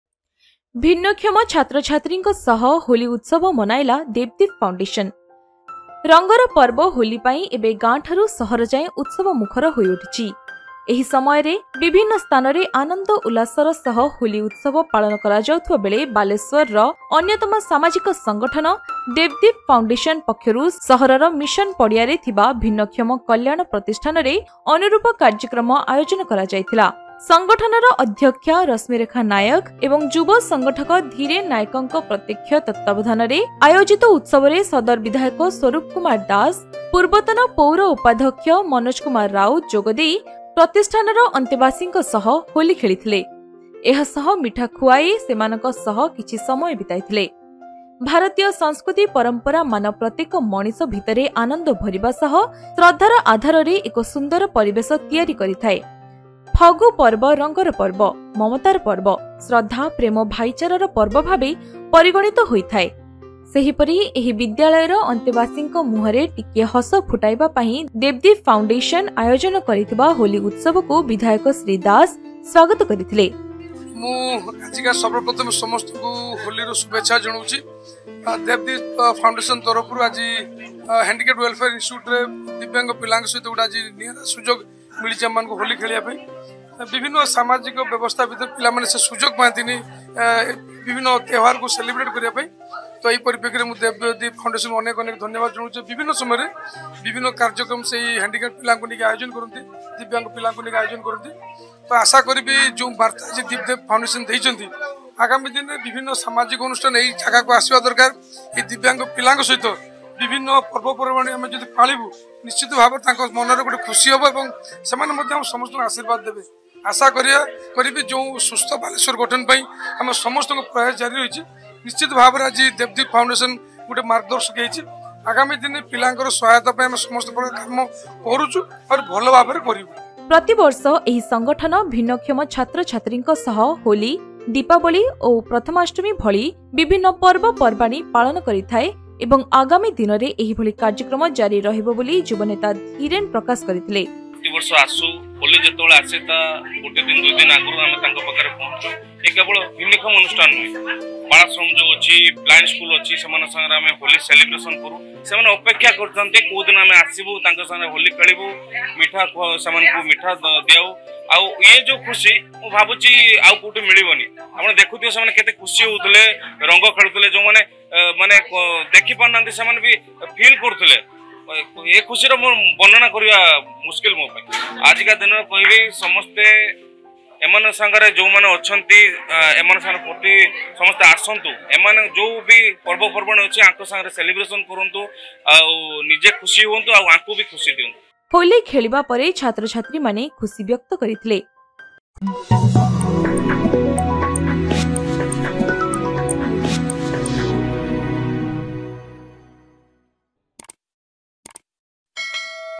ଭିନ୍ନକ୍ଷମ ଛାତ୍ରଛାତ୍ରୀଙ୍କ ସହ ହୋଲି ଉତ୍ସବ ମନାଇଲା ଦେବଦୀପ ଫାଉଣ୍ଡେସନ, ବାଲେଶ୍ୱର
For the festival of colors, Holi is now celebrated from village to city. At this time, while Holi was being celebrated with joy and happiness in various places, Devdeep Foundation, one of the social organizations of Baltimore, organized a corresponding program at the differently-abled welfare institute in the Mission area of the city. Sadar MLA Swarup Kumar Das, former Deputy Mayor Manoj Kumar Raut joined in the celebration organized under the direct supervision of Rashmirekha Naik and youth organizer Dhiren Naik and played Holi with the inmates of the institution. He also ate sweets and spent some time with them. Indian cultural traditions fill every human being with joy and create a beautiful environment based on reverence. Fagu festival is considered as the festival of colors, the festival of motherhood, the festival of devotion, love and brotherhood. Similarly, MLA Mr. Das welcomed the Holi celebrations organized by Devdeep Foundation to bring a smile to the faces of the inmates of this school. Every year, this organization celebrates various festivals like Holi, Diwali and Prathamstami with disabled students and said that similar programs will continue in the coming days. After playing Holi, the students expressed their happiness